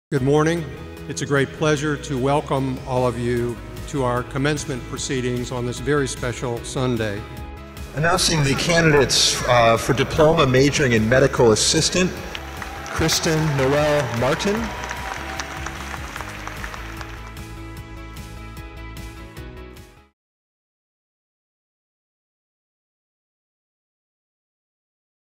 2014 Graduation for Medical Assistant Diploma (KNM)
Congrats to our graduate! This special moment was captured at the Graduation Ceremony for Mount Washington College's Medical Assistant Diploma on May 18, 2014.